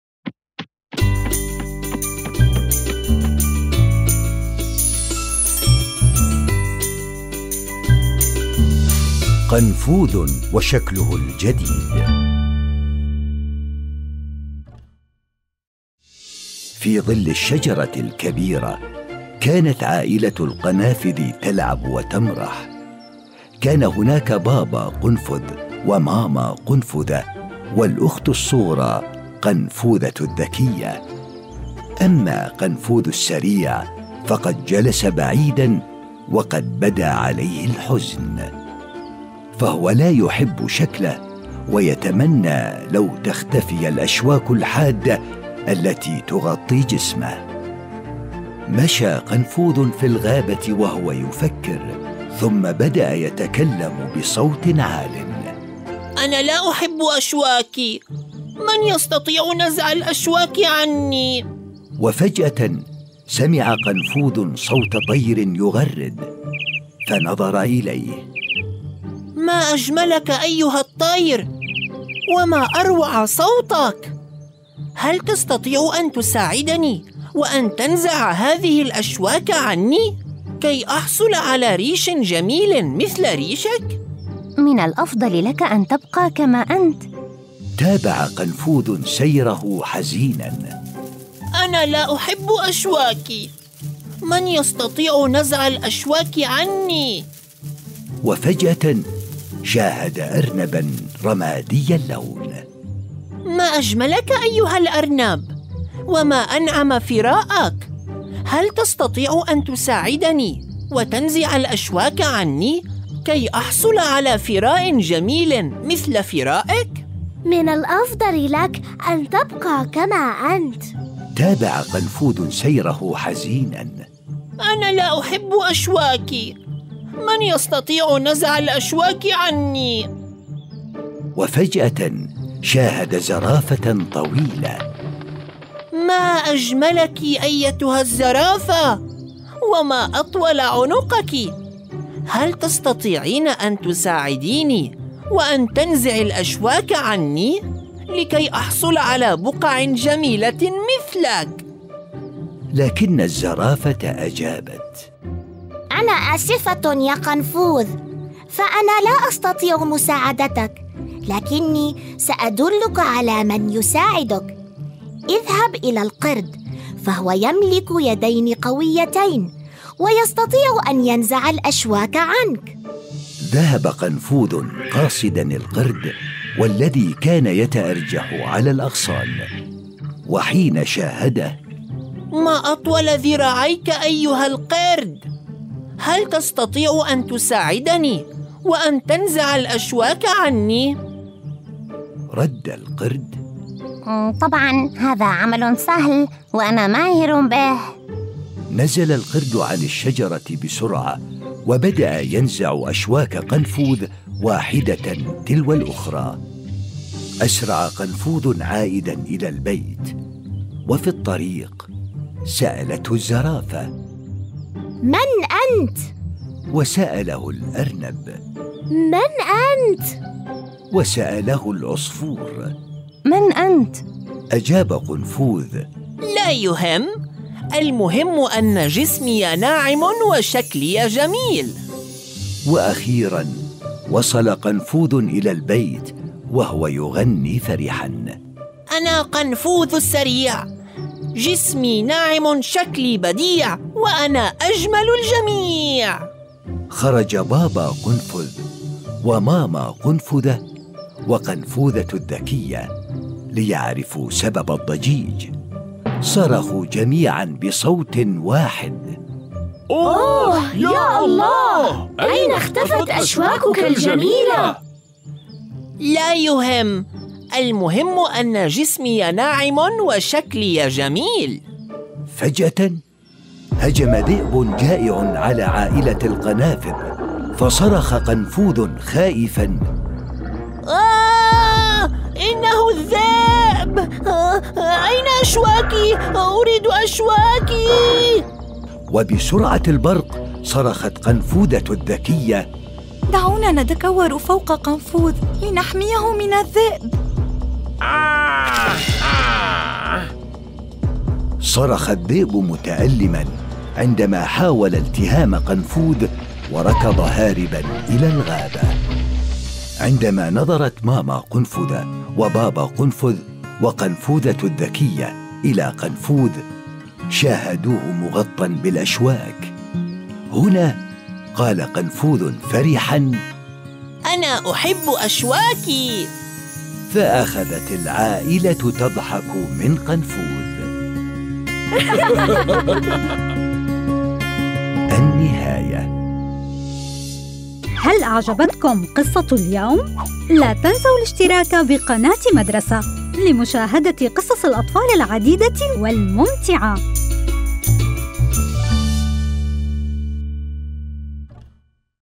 قنفوذ وشكله الجديد | قصص مدرسة
في أحد الأيام يجد قنفوذ أنه لا يحب أشواكه ويرغب في الحصول على جسم ناعم ويبدأ في البحث عن من يساعده في ذلك